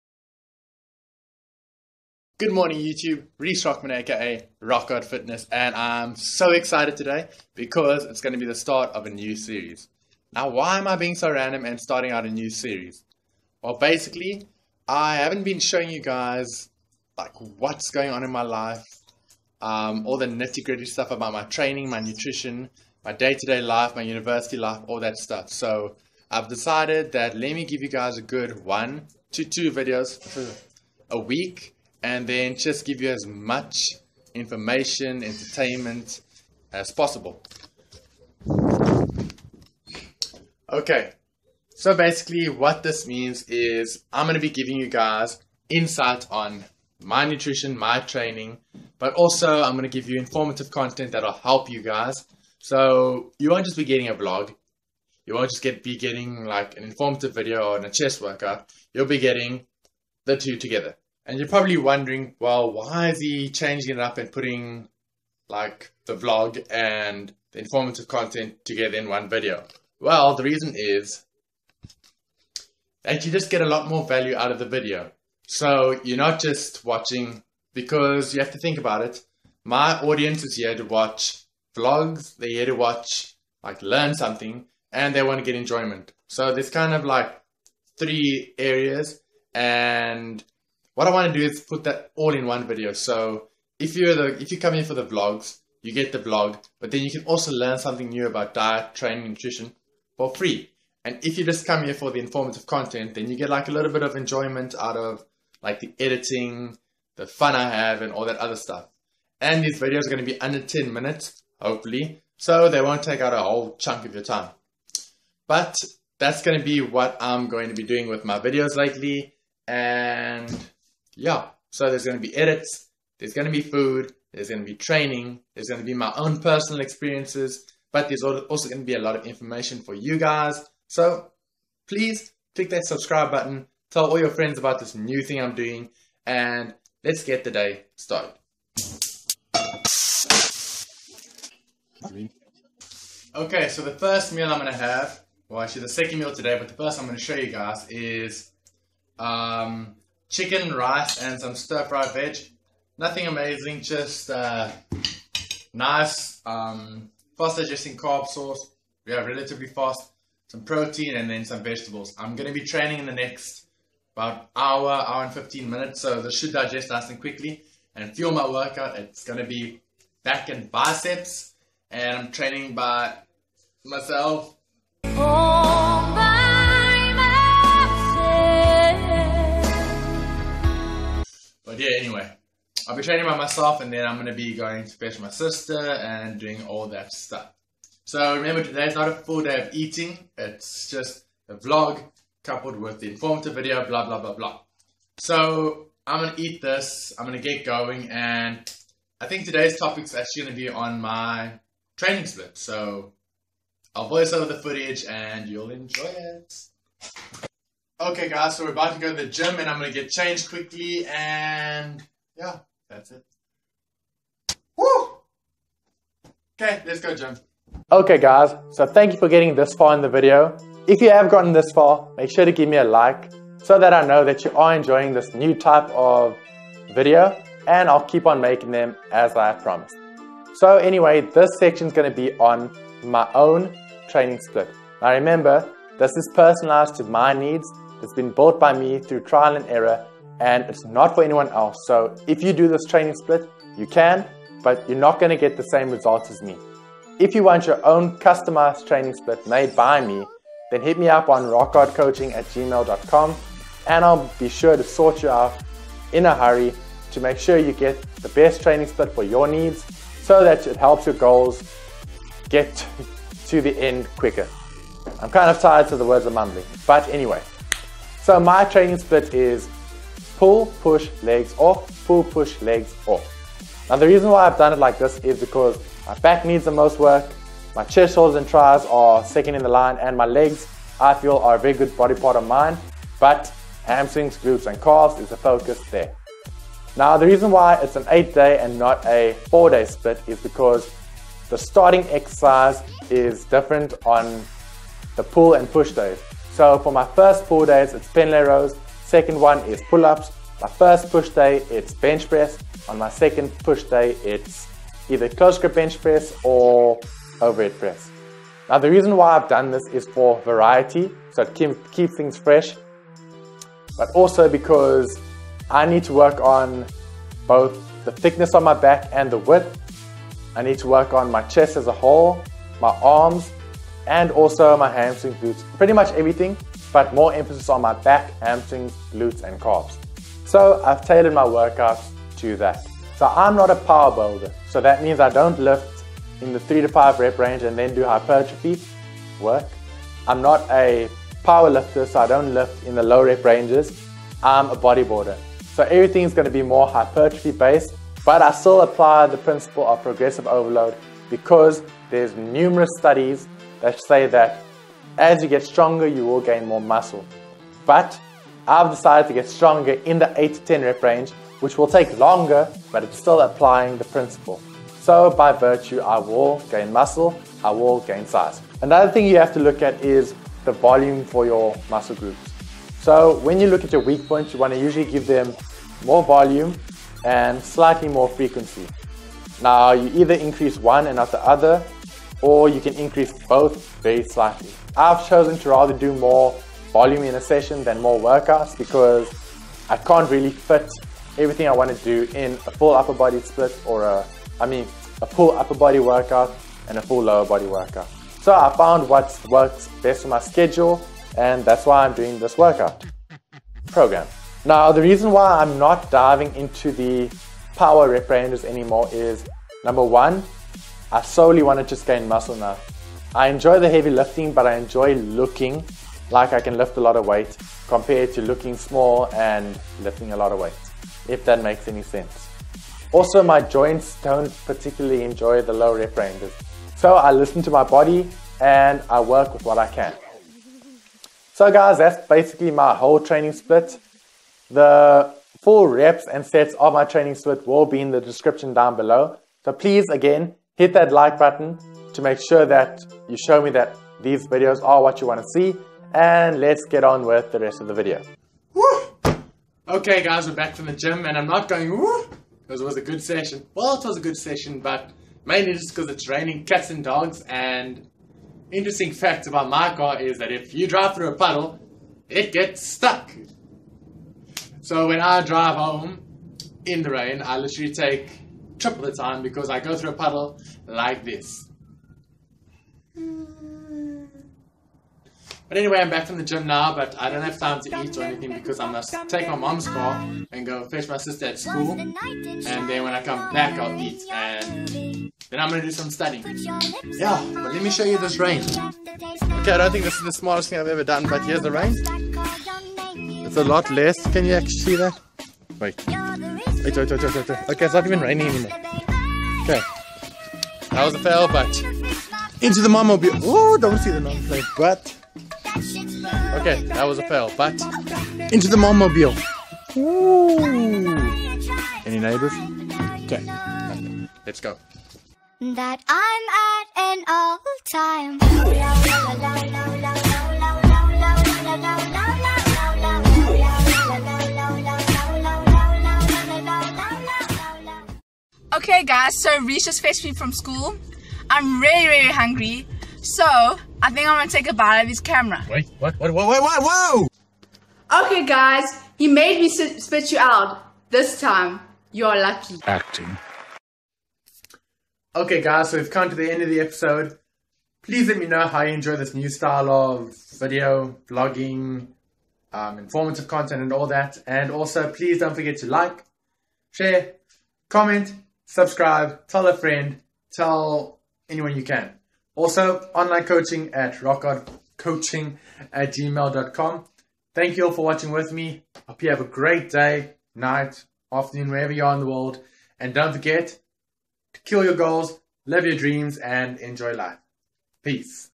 THE NEW SERIES EP. 1: MY PERSONAL PULL/PUSH/LEGS SPLIT
Day 1: Pull (Pendlay Row Focused)

Pendlay Row: 3 sets 8-10 reps
1 Arm Lat Pulldown: 3 sets 10-12 reps
Close Grip Cable Rows: 3 sets 10-12 reps
T Bar Row: 3 sets 8-10 reps
EZ Bar Preacher Curls: 3 sets 8-10 reps
Dumbbell Hammer Curls: 3 sets 10-12 reps
Cable Reverse Curls: 2 sets 12-15 reps

Day 2: Push (Bench Focused)
Flat Bench Press: 3 sets 8-10 reps
Incline Dumbbell Press: 3 sets 8-10 reps
Cable Flies: 3 sets 10-15 reps
1 Arm Dumbbell Extention: 4 sets 10-12 reps

Day 4: Rest

Day 5: Pull (Pull Up Focused)

Wide Grip Pull Ups: 3 sets 8-10 reps
2 Arm Bent Over Dumbbell Rows: 3 sets 8-10 reps
Close Grip Pulldowns: 3 sets 10-12 reps
Arms Flared Cable Row (Rhomboids): 3 sets 10-12 reps
Barbell Curls: 3 sets 8-10 reps
1 Arm Cable Curls: 3 sets 10-12 reps
Overhand EZ Bar Curls: 2 sets 12-15 reps

Day 6: Push (Overhead Press Focused)

Overhead Press/ Close Grip Bench Press: 3 sets 8-10 reps
Flat Dumbbell Press: 4 sets 8-10 reps
Incline Paused Bench Press: 3 sets 8-10 reps
1 Arm Dumbbell Extention: 4 sets 10-12 reps
Tricep Pushdowns: 4 sets 12-15 reps
Chest Supported Rear Delt Fly: 4 sets 10-12 reps
Cable Side Raises: 3 sets 10-12 reps

Day 7: Legs

Day 8: Rest